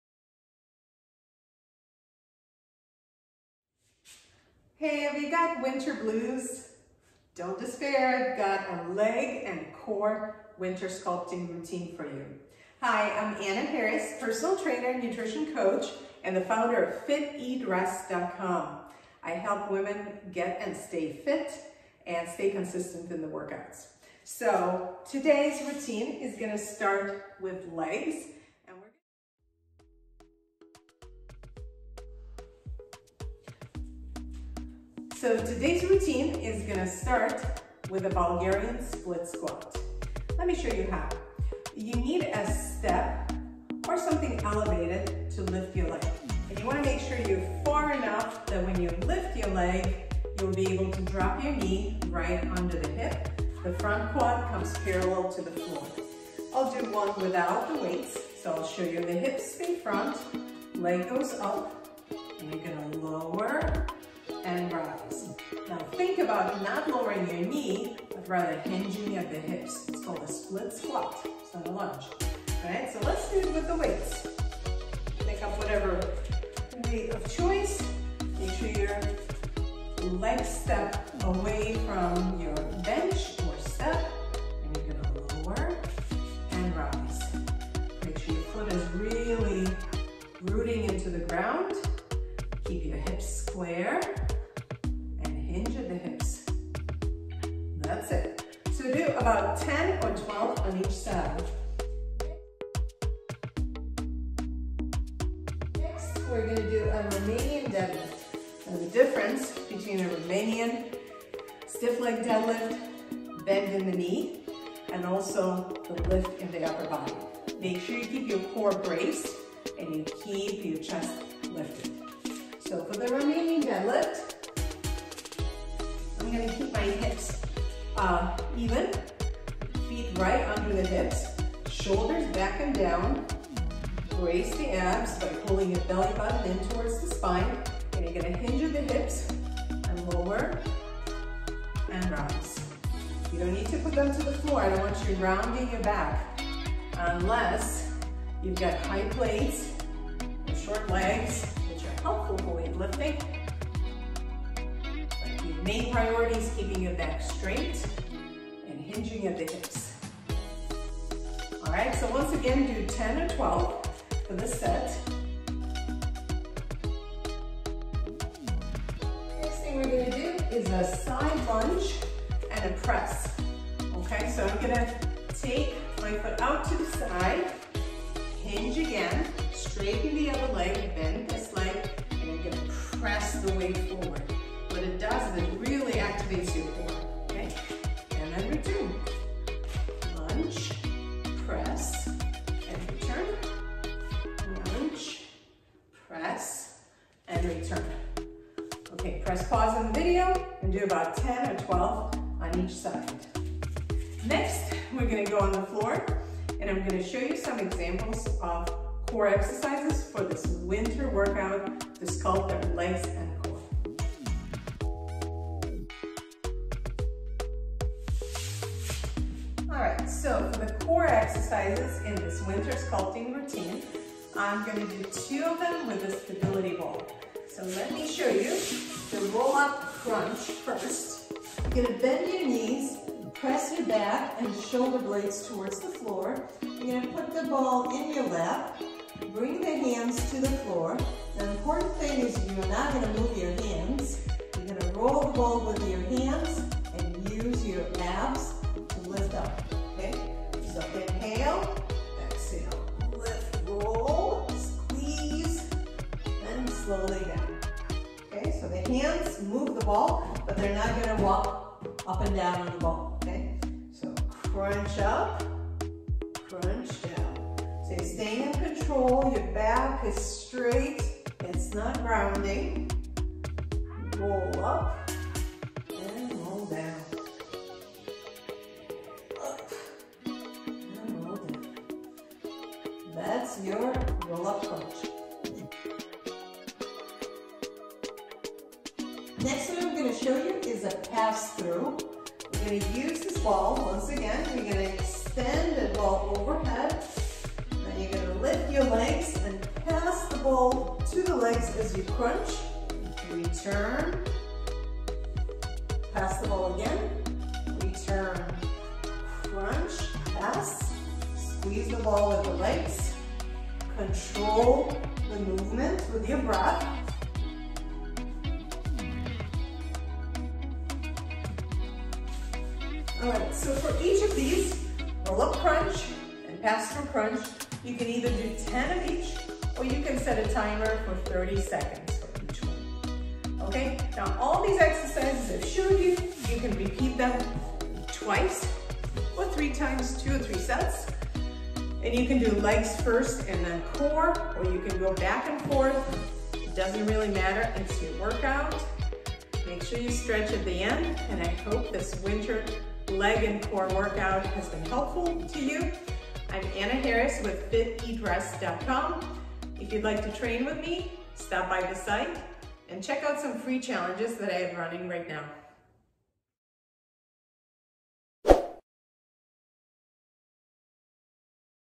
Leg & Core Winter Sculpting Routine
Sculpt your legs and core with this winter workout routine.

Find more fitness tips and workout challenges at FitEatRest.com